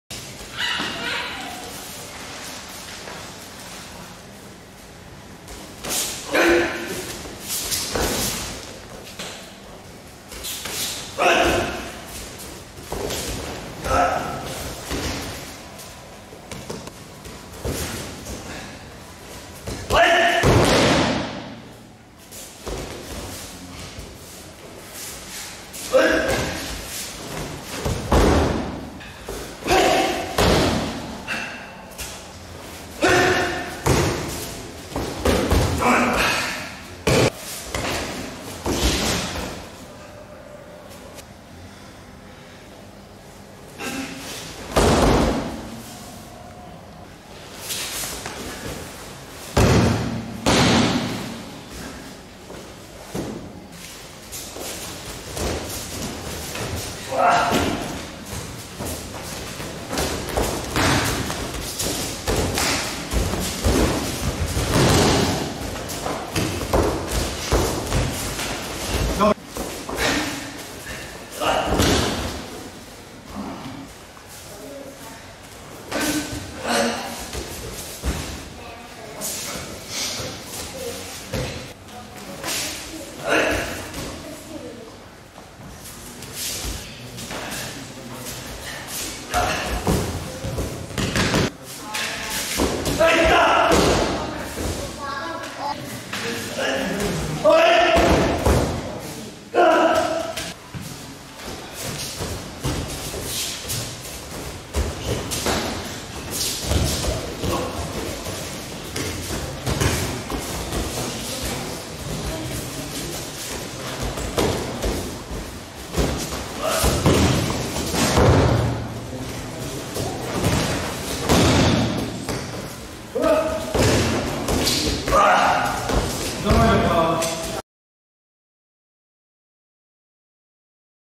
Slobodan Vukovic 1.kyu Braon pojas | Internacionalna aikido akademija
Na Instruktorskom aikido seminaru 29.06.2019.g, koji je vodio Osnivac iInternacionalne aikido akademije Bratislav Stajic, Slobodan Vukovic, polozio je za 1. kyu Braon pojas i stekao zvanje Majstorskog kandidata.

Komisija za polaganje:

Predsednik komisije

Bratislav Stajic, 9.Dan (Srbija)

Clanovi komisije

Dejan Zuza, 6.Dan (Srbija)
Milorad Vujovic, 6.Dan (Srbija)
Uros Jezernik, 5.Dan (Slovenija)